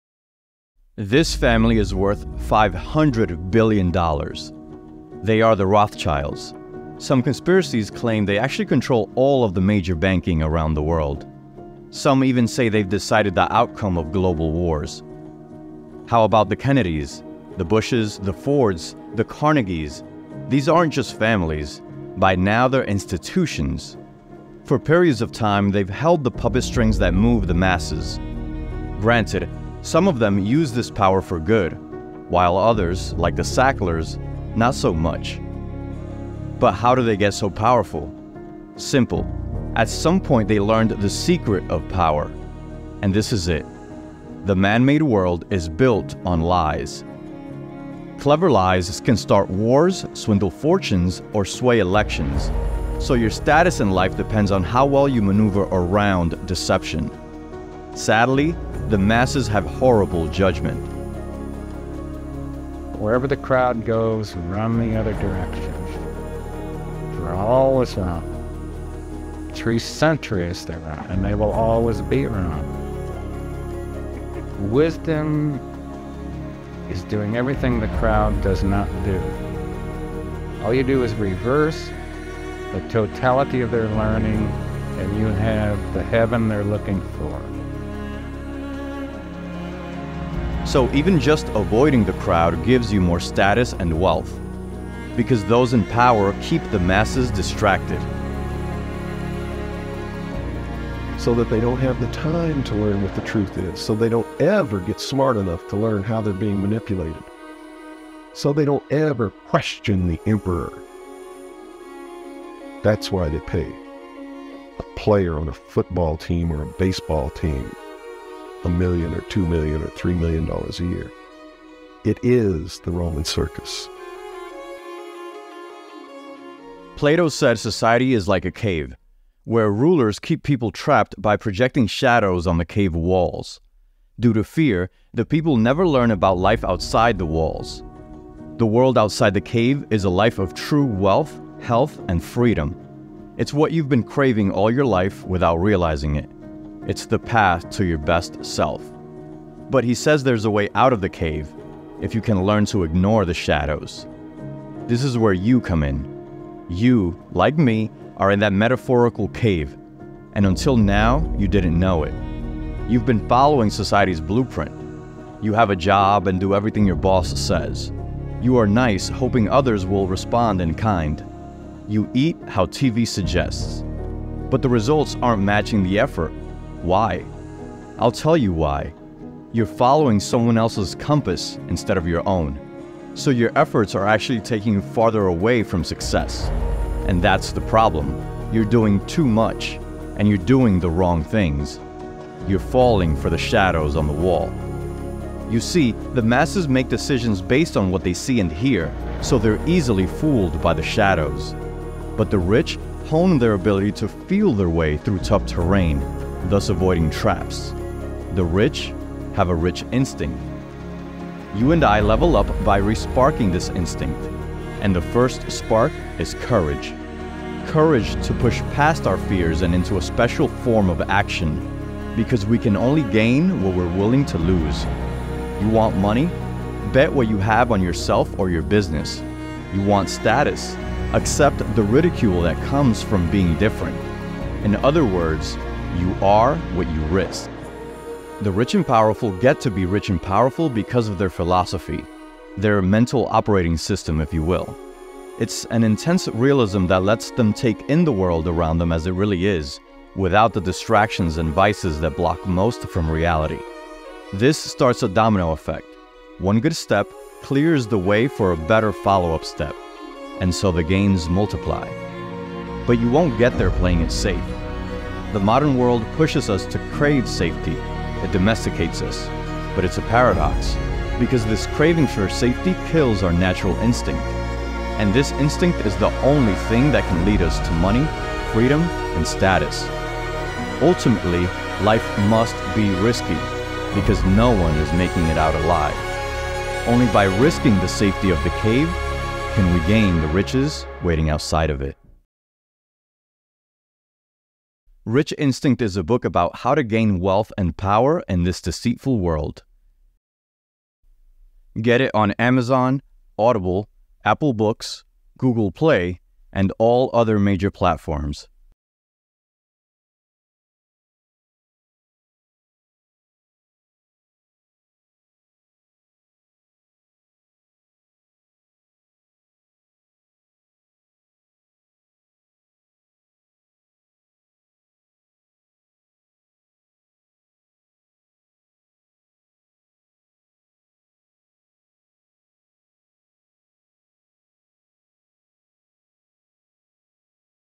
Rich Instinct: What Sets Apart the Powerful
The wealthy are different from you and I. Through nature or nurture they've been able to hone their skill at spotting the lies of the world. And this is what makes them rich. Families like the Rothschilds, the Kennedys, and the Bushes do things in a certain way that brings them their fortune.

Same goes for the self-made billionaires like Jeff Bezos and Elon Musk. They've internalized something the rest of us haven't. And this lesson goes all the way back to the times of Plato and the Stoics.

Learn the tips and tricks to gain power in this deceitful world.

References:
Charles Bukowski (poet and novelist) interview "Wherever the crowd goes, run the other direction."
Milton William Cooper (author of Behold a Pale Horse) interview "The Roman circus"